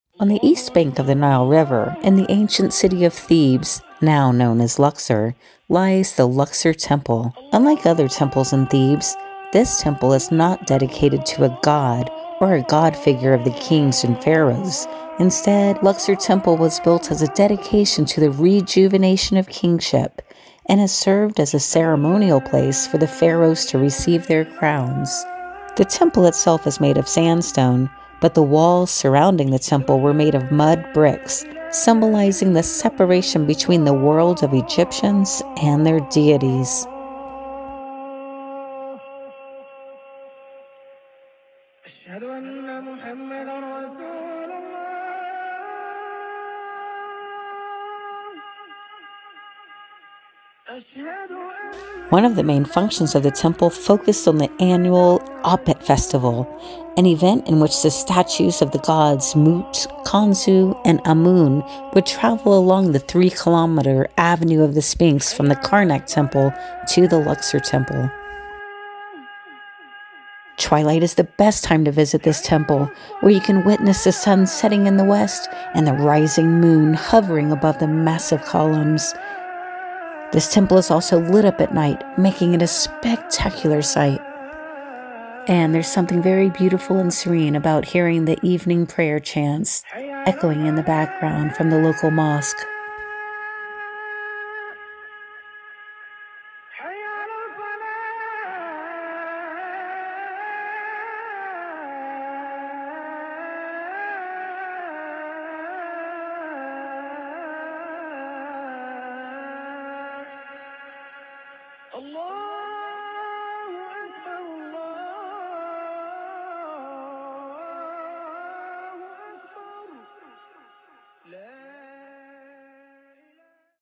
Experience the Grandeur of Luxor Temple
Luxor Temple is best seen at twilight. Watch this video to see why.